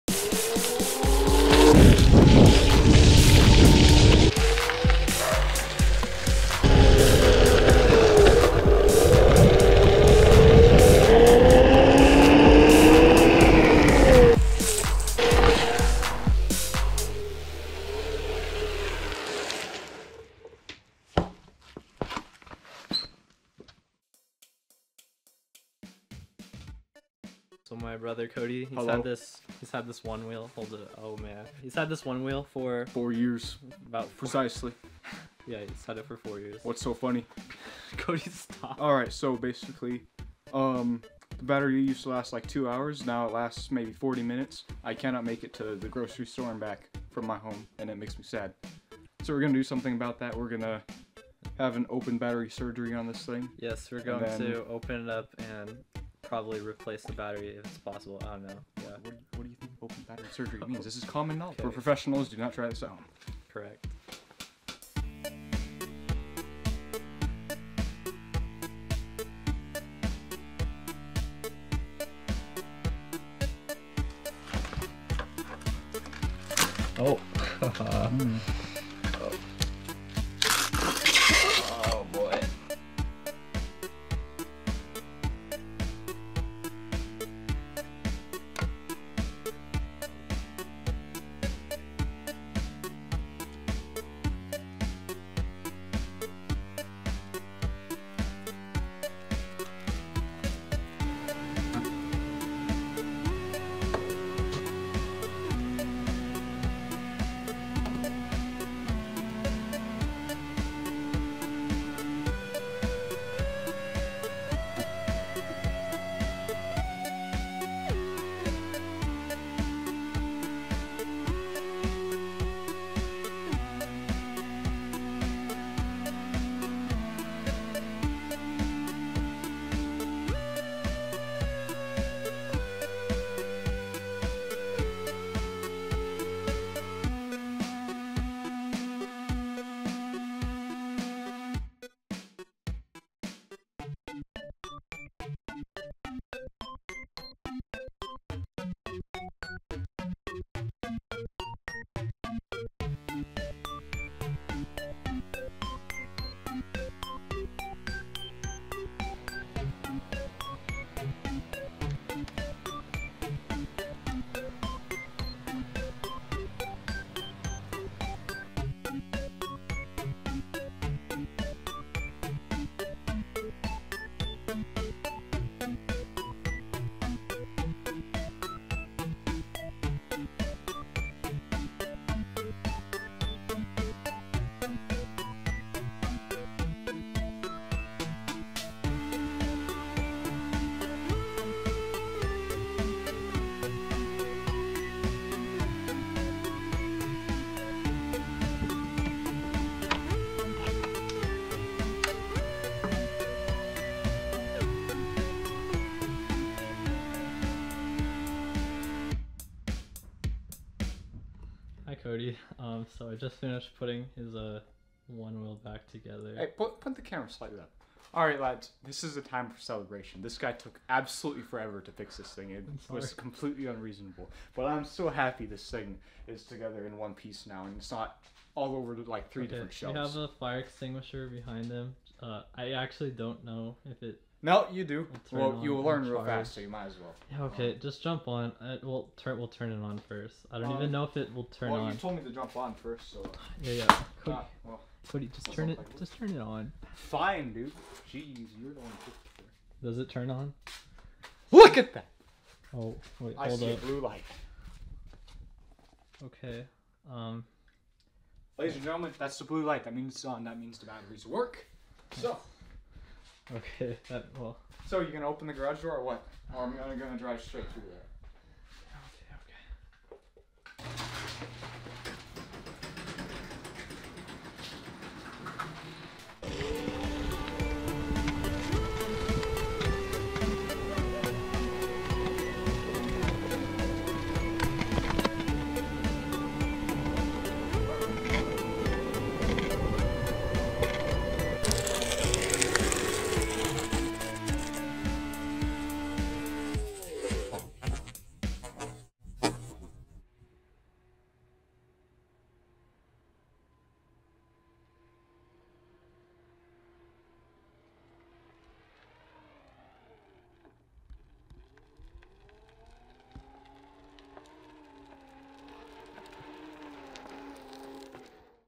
Onewheel Battery Replacement
In this video, I replace the batteries in my little brother's Onewheel Original. The battery life had degraded severely in the four years since my brother got it. After replacing the batteries, the Onewheel behaved like it was brand new and regained its full range.

The Onewheel Original has 16 ANR26650 lithium-ion batteries in series. I ordered replacements through

This project consisted of disassembling the Onewheel, assembling a replacement battery pack, soldering the charge control and power wires from the old battery pack onto the new one, and sealing up the battery and putting the Onewheel back together. I used a Sunkko 709A spot welder and 0.2mm thick, 8mm wide nickel strips (both bought on Amazon) to assemble the battery pack, and a lot of Kapton and electrical tape to seal it up.

Working with lithium-ion batteries can be dangerous. You probably shouldn't try this at home if you don't know what you are doing... kinda like me since this was my first time assembling a battery pack :)

Hope y'alls learned something new!